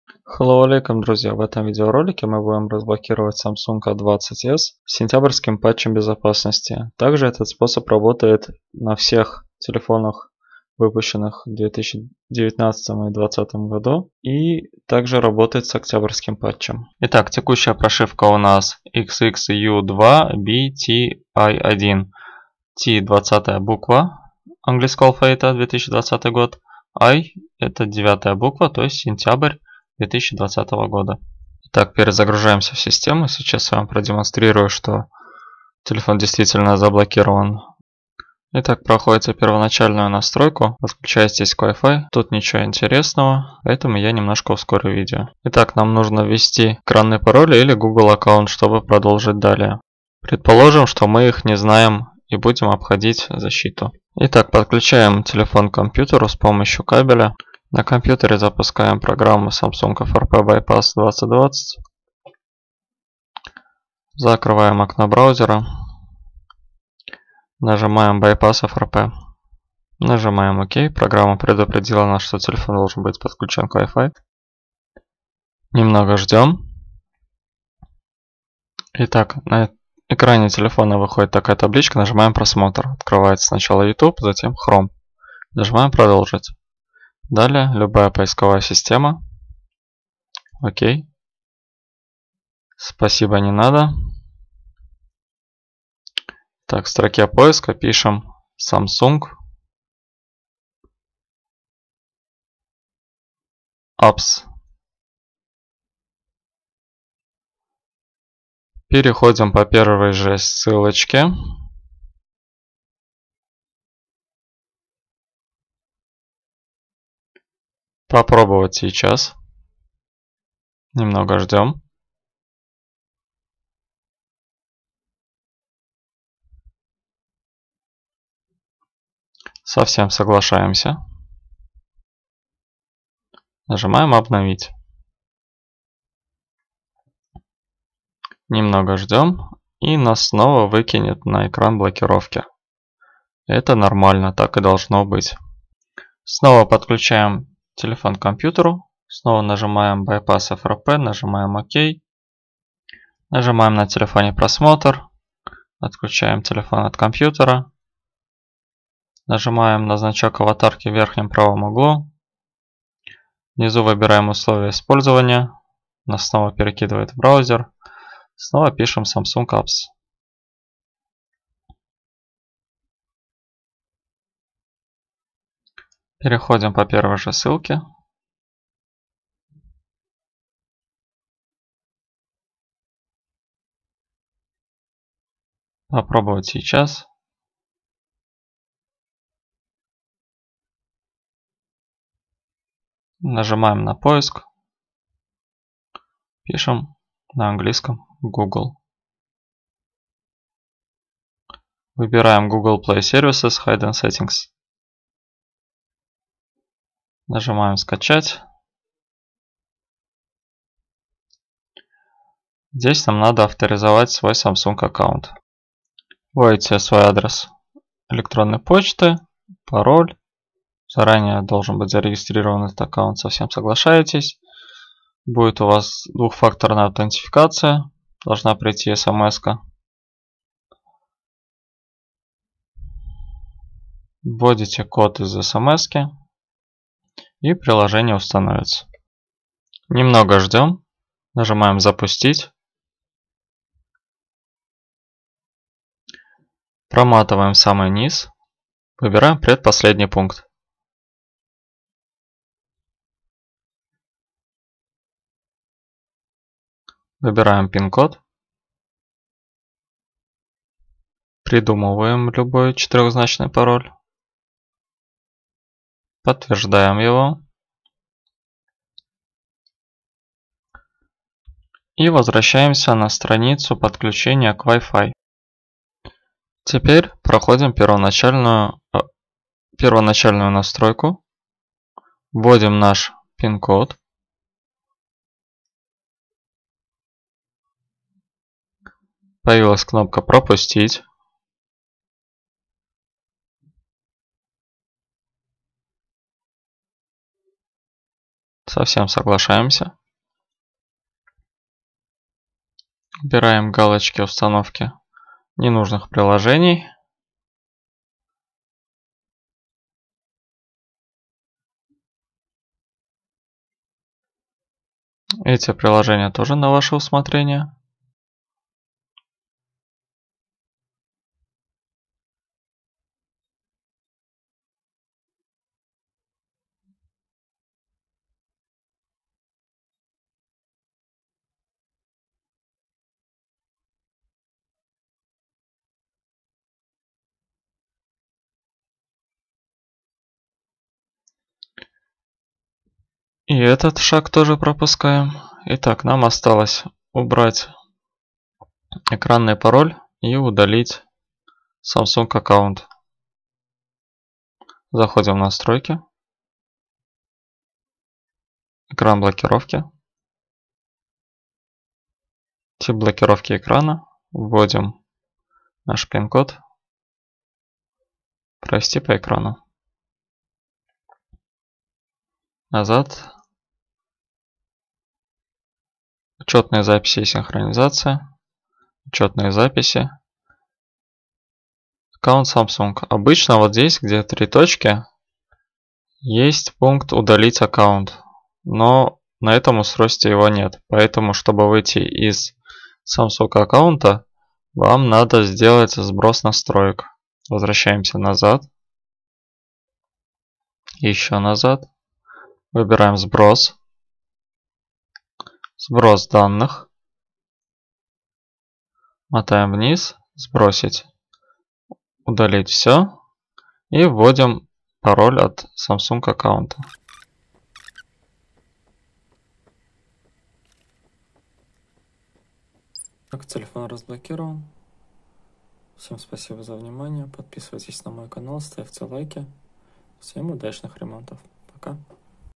Samsung A20s frp
Обходим блокировку по гугл аккаунту на смартфонах samsung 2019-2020 года с Android9 и 10 (патч безопасности сентябрь, октябрь 2020)
Ссылка на программу в закрепленном комментарии.
Всем удачных ремонтов